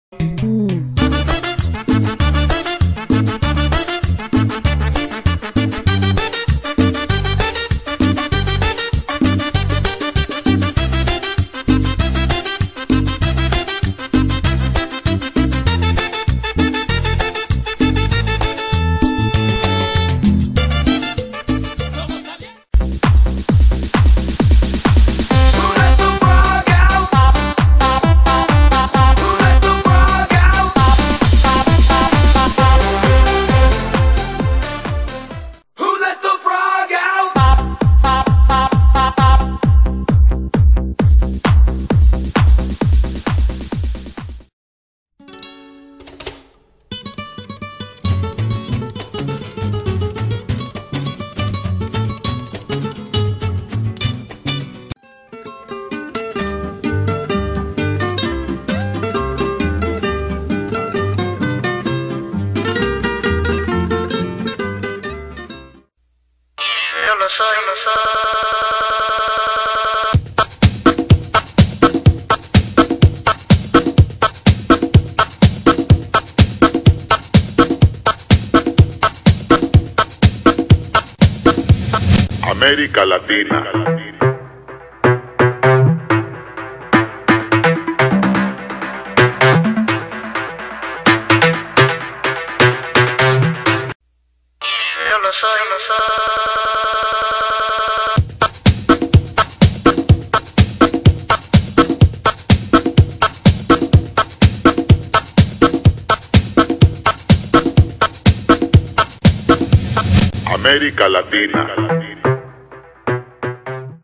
Pollada, Festival de musica, danzas y torneo de tiro al sapo
El festival se realizara  en 150 Milton St  Broklyn NY  11222 donde la familia podra disfrutar de danzas andinas, marinera nortena, festejo con los mejores bailarines de PERUINCAFOLK, las mejores musicas bailables del Peru con el mejor DJ "Tumba la Casa", juegos de tiro al sapo ,ademas degustaras la tradicional pollada y bebidas.